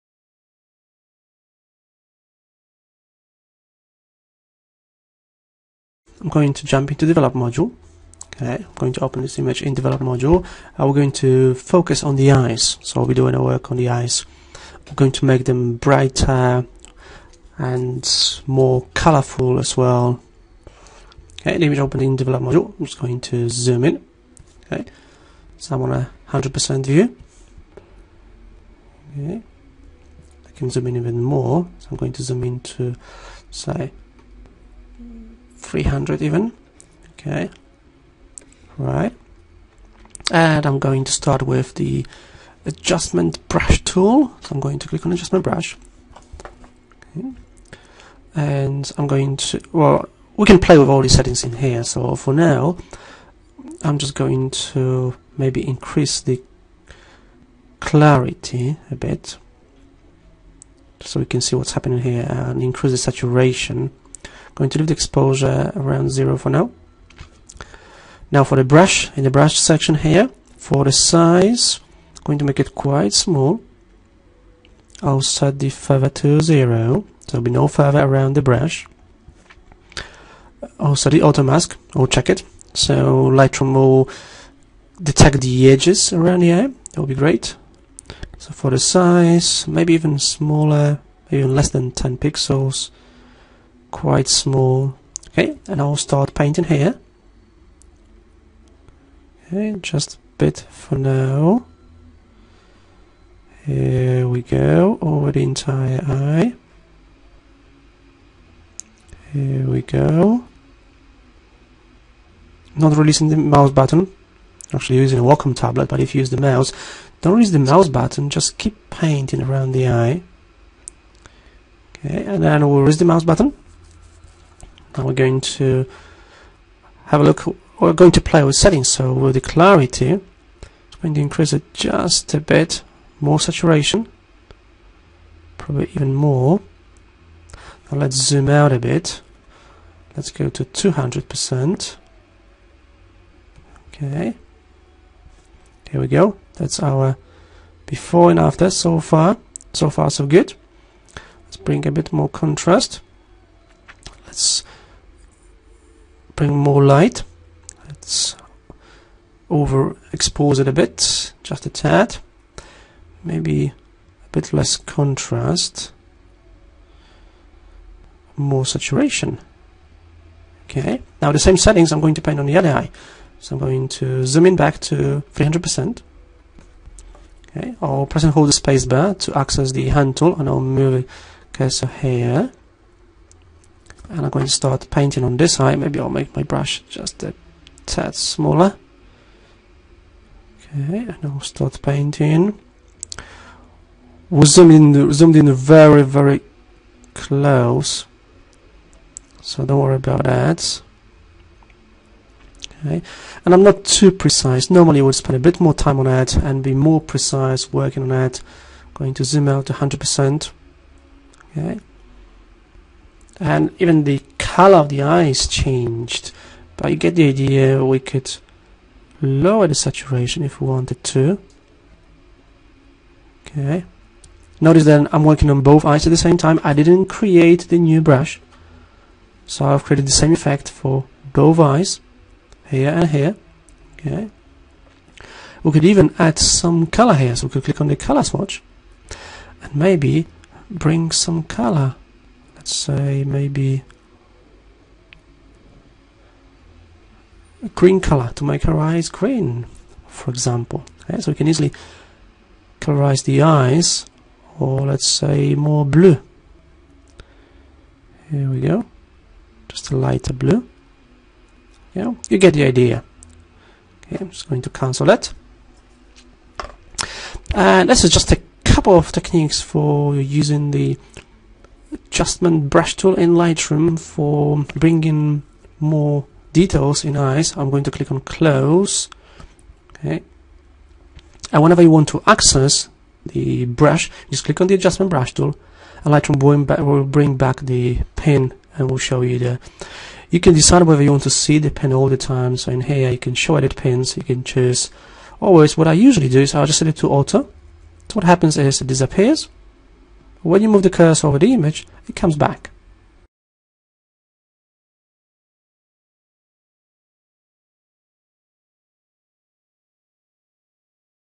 Eyes retouching in Adobe Photoshop Lightroom 4
Learn some new techniques of eyes retouching in Adobe Photoshop Lightroom 4, with Marek Mularczyk from SaiTraining.co.uk - one of the UK's leading specialists in Adobe Certified Training.